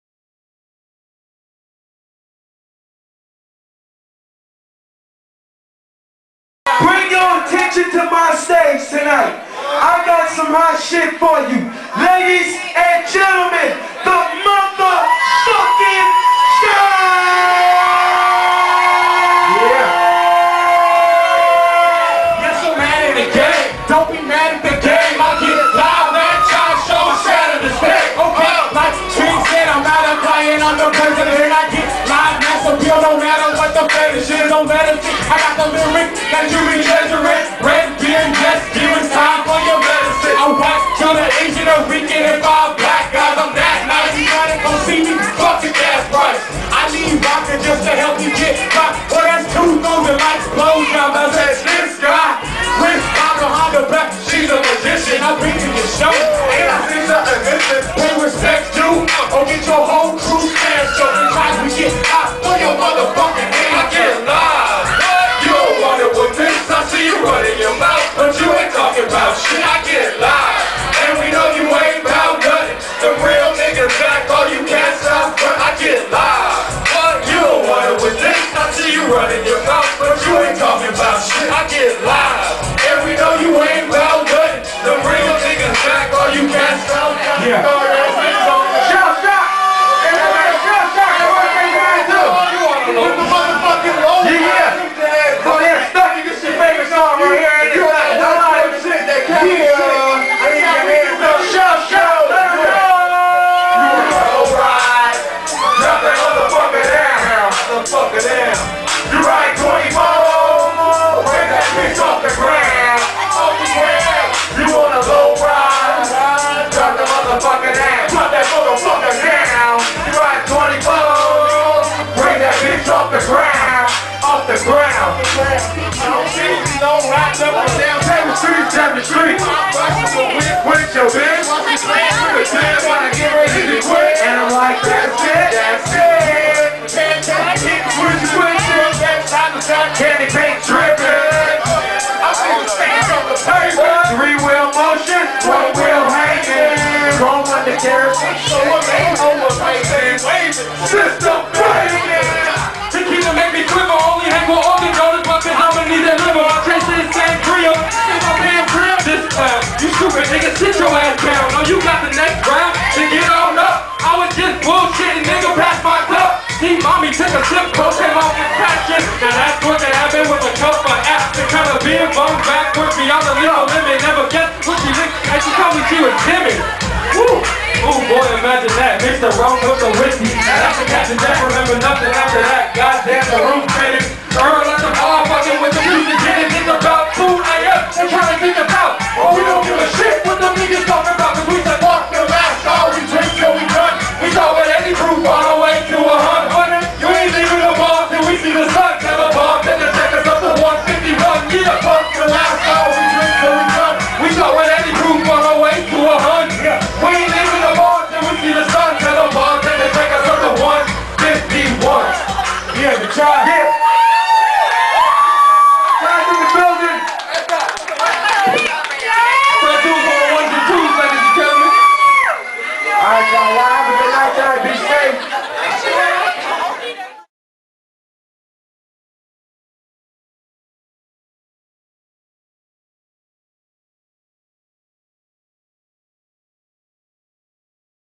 The Chise at Martini Blues
The Chise perform at Martini Blues in Huntington Beach, CA.